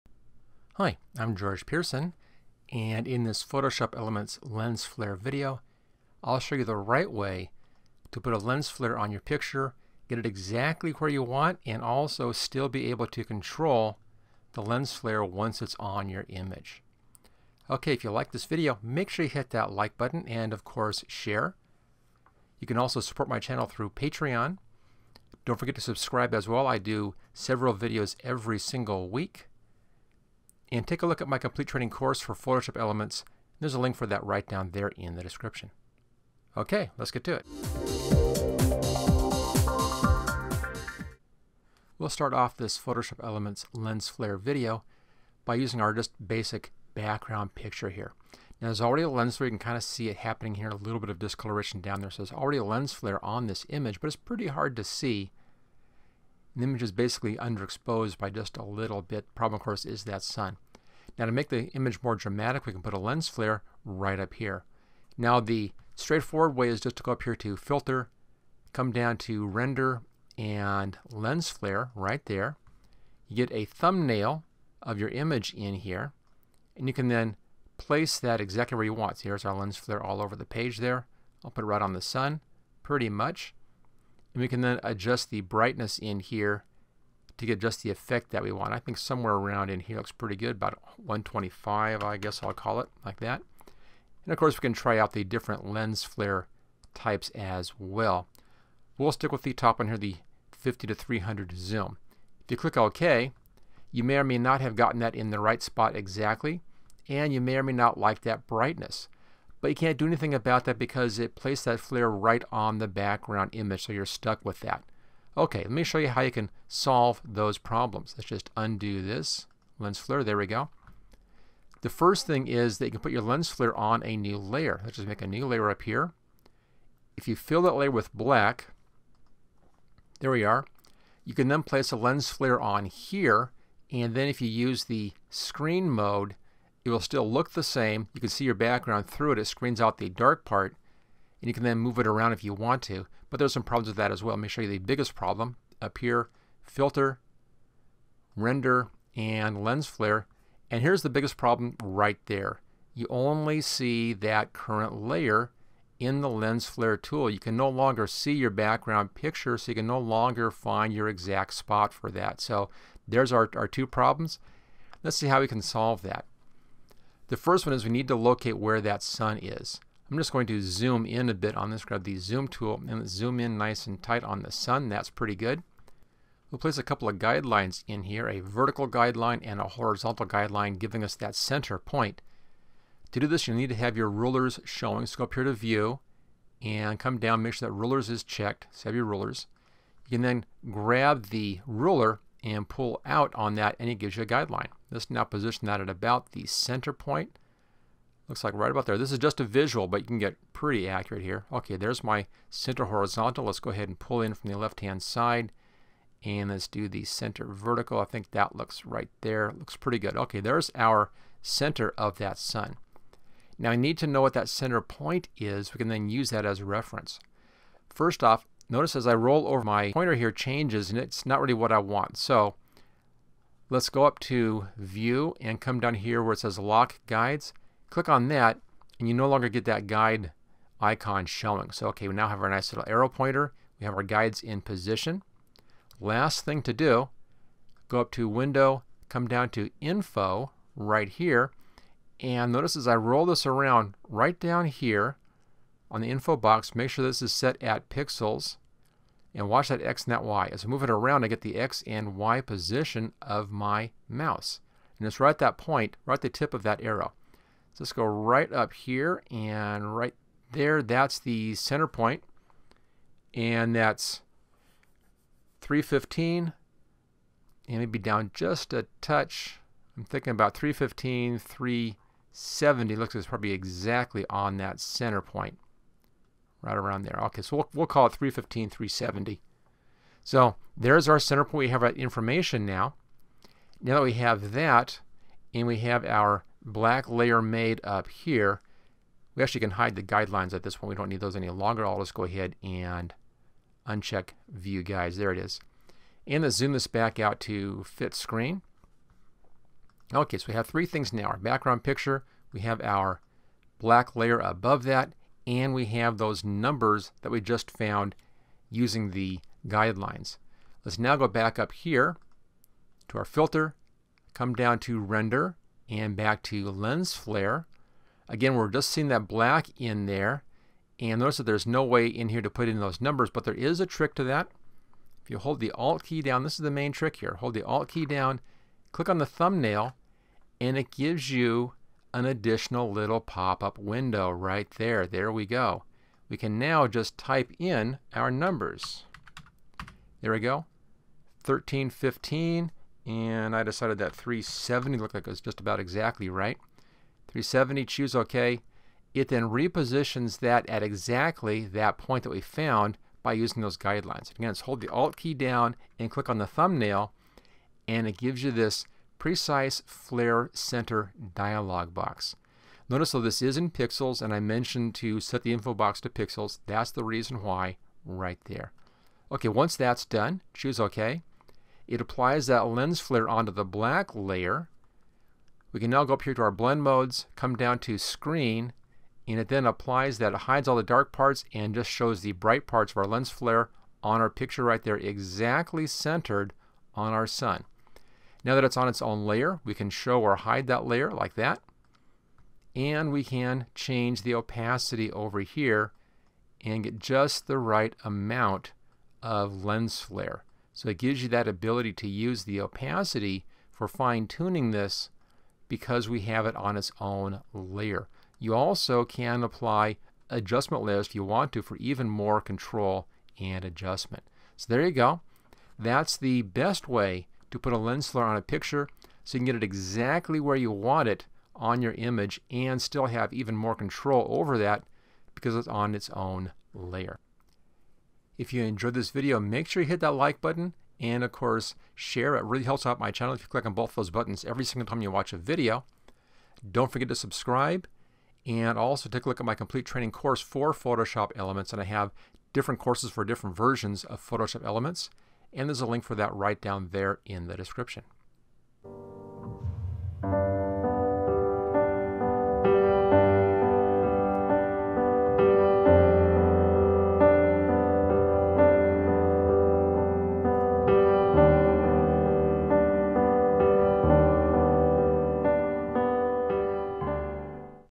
How You Can Use the Photoshop Elements Lens Flare Filter Correctly Tutorial
Photoshop Elements Lens Flare Filter. I show you how the Lens Flare Filter works and a couple of neat Photoshop Elements tricks to get the most control out of the lens flare effect. The Photoshop Elements Lens Flare Filter is a great way to add some excitement to a picture. But there is a wrong way and a right way to use this filter. In this video  In this how to project I show you a little known trick with the Lens Flare filter.